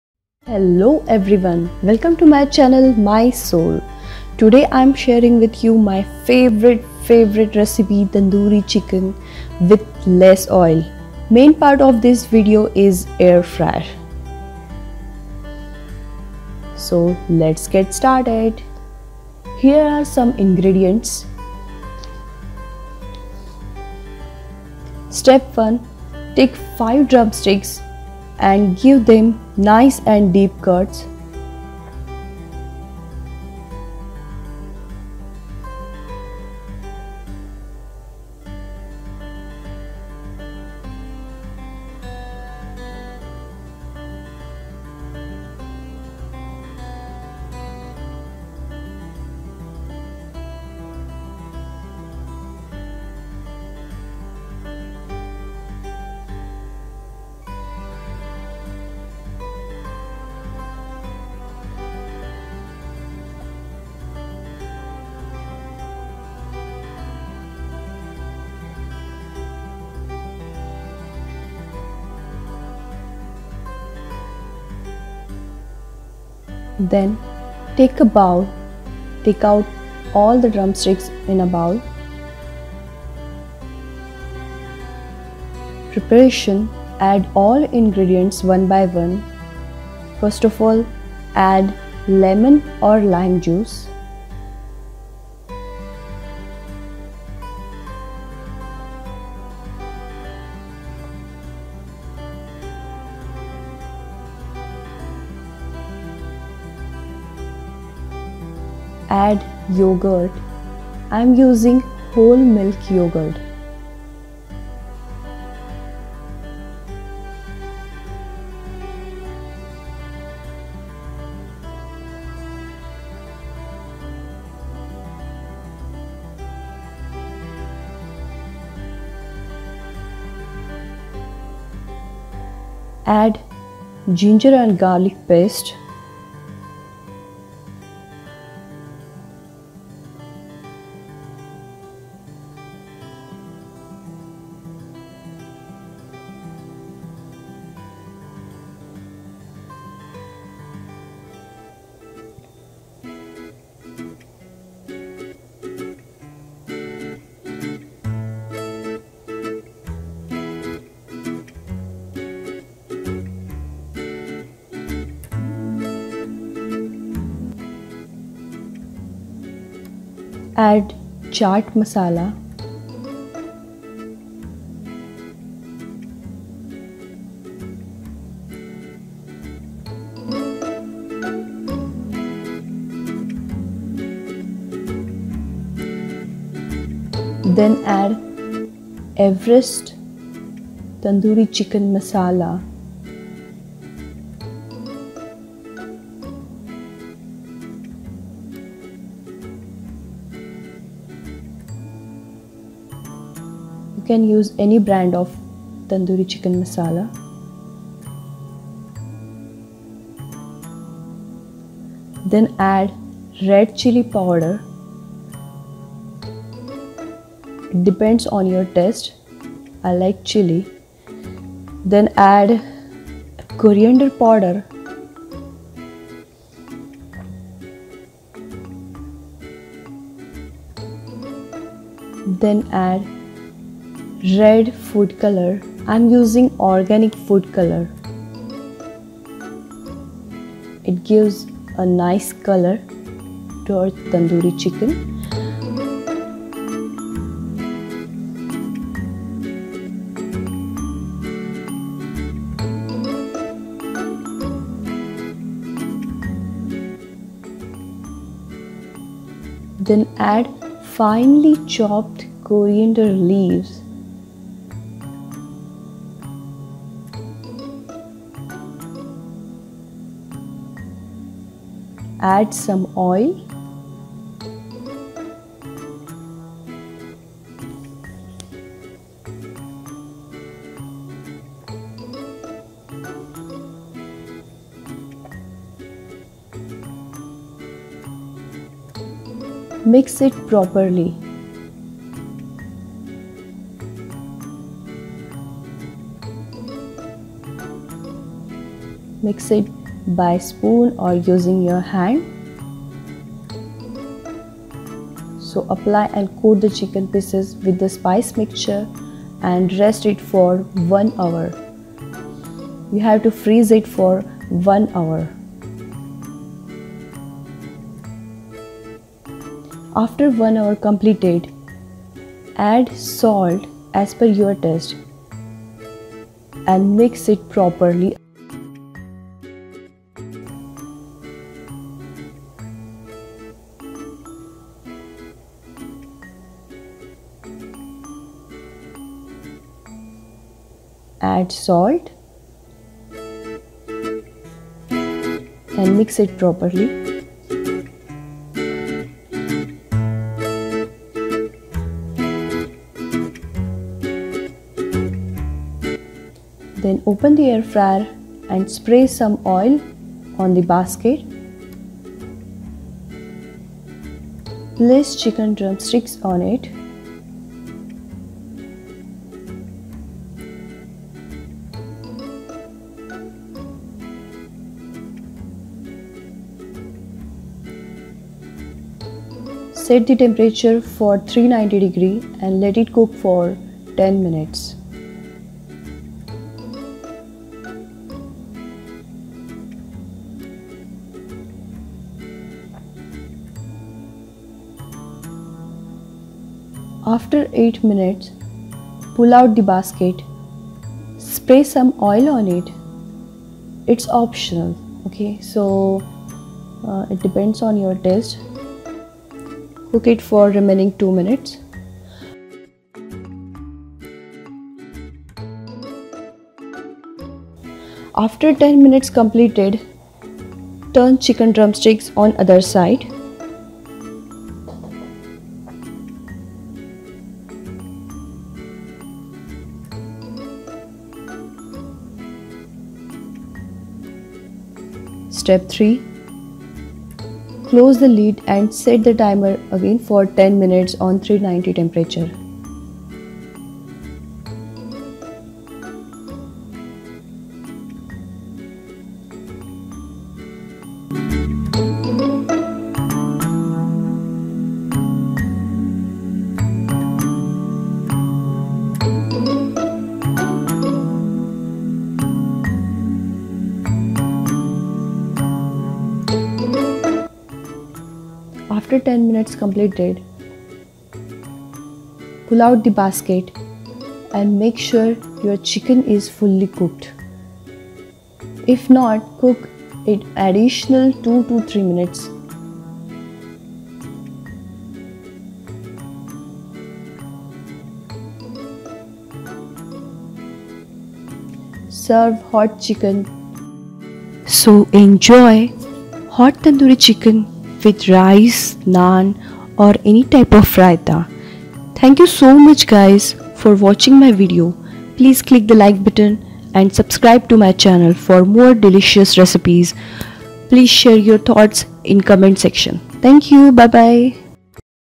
Healthy And Delicious Chicken Tandoori Recipe In Philips Airfryer
Chicken Tandoori
ChickenTandoori is a popular Indian dish made with chicken drumsticks marinated with spices and  cooked in the Phillips Air fryer using very little oil hence it makes a healthy and nutritious dish.
About Recipe
Kodi vepudu, varuval, Muragira mansa Bhaja
How to make Chicken Tandoori Recipe in Philips Airfryer by Indian Food with Rashmi.
Prep time
5 mins Cook time
20 mins Total time
25 mins Chicken Tandoori Recipe in Philips Airfryer
Author : Rashmi
Main Ingredient : Chicken drumsticks
Servings : 3 persons

Ingredients used in Chicken Tandoori Recipe in Philips Airfryer
1.  Chicken drumsticks - 5 numbers.
2.  Salt - as per taste.
3.  Ginger paste- 1 tbsp.
4. garlic paste - 1 tbsp.
5. Red Chili powder - 1 tsp.
6. Everest tandoori masala - 1 tbsp.
7. Yogurt- 1/4 cup.
8.  Coriander powder - 1 tsp.
9. Red food color - 1/2 tsp.
10.  Lime juice - 1 1/2 tbsp.
11. Chat masala - 1 tsp.
12.  Oil - 1 tbsp.
13. Some cilantro.
Method:
1.Take the chicken drumsticks and give few cuts over the drumsticks.
2.Take chicken drumsticks in a bowl and add  ginger garlic paste, red chili powder, tandoori masala , chat masala , red food color, coriander powder,  lime juice , cilantro, and  yogurt. Mix all the ingredients well.
3.Apply and coat the chicken pieces with the spice mixture and freeze it for 0ne hour.
4.Open the lid of philips airfryer and spray some oil . Place the chicken drumsticks one by one and close the lid. Set the temperature on 390 for 10 min.
5.After 8 min, spray some oil (optional). Close the lid to complete the 10 min process(to complete remaining 2 min).
6.Then turn the chicken on other side and again set the temperature on 390 for 10 min. Note: If required cook it for extra 2-3 min.
7.After that open the lid , remove the chicken  and hot serve with bread or naan or roti or rice.